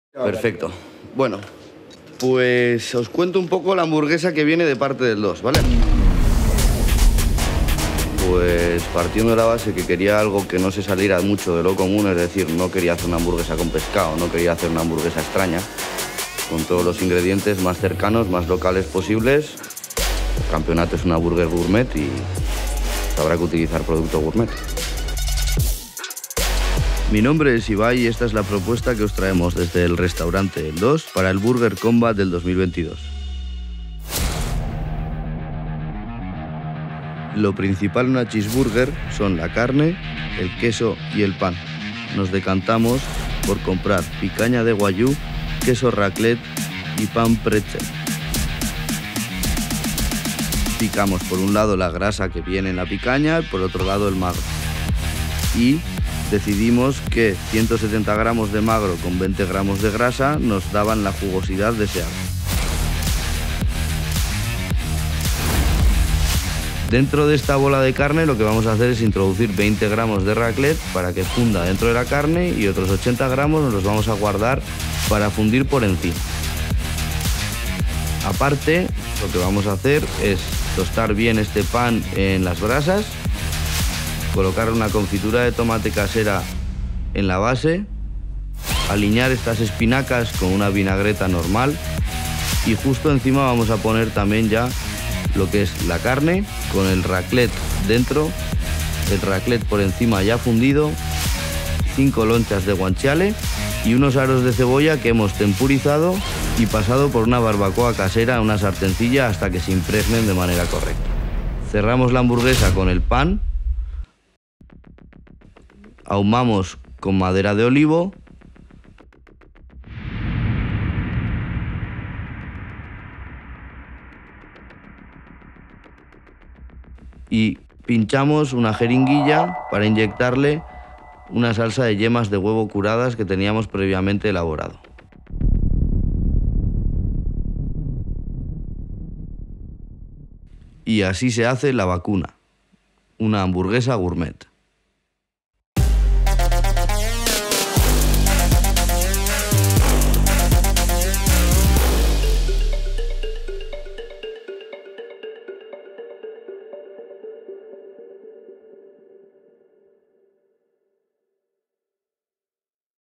La Vakuna - BURGER COMBAT Euskadi 2022
Creado para El Escandallo
Dirección y montaje - Alex García
Codirector y diseño de sonido - Jon "KJ" Karlos
Producción - Cesar Romero
DoP Auxiliar - Julen Agirre
Grabado en Vitoria-Gasteiz